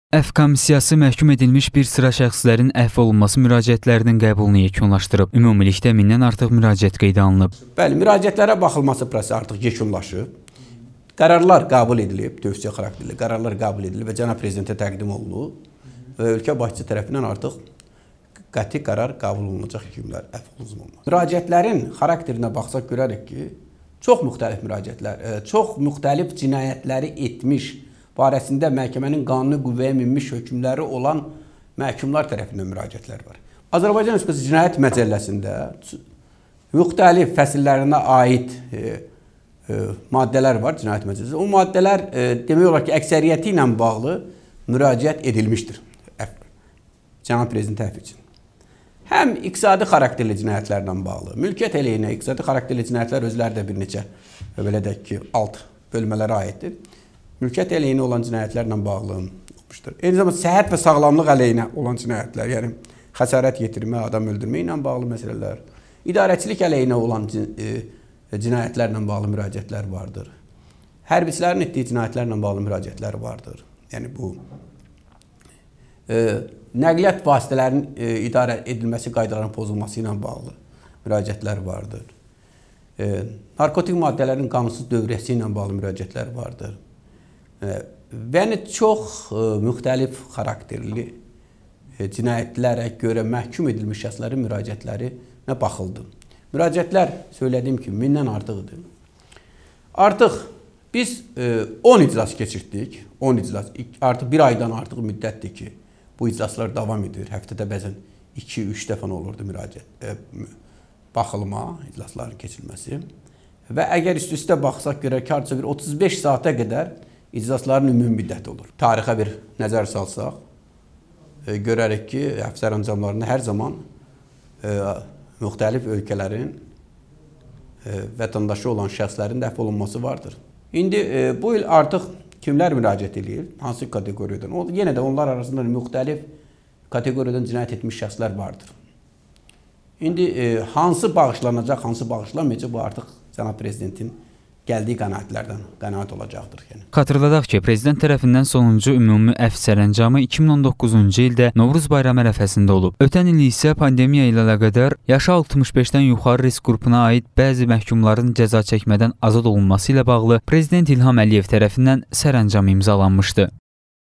Əfv üçün mindən artıq müraciət var
Əfv komissiyasi müraciətlərin qəbulunu yekunlaşdırıb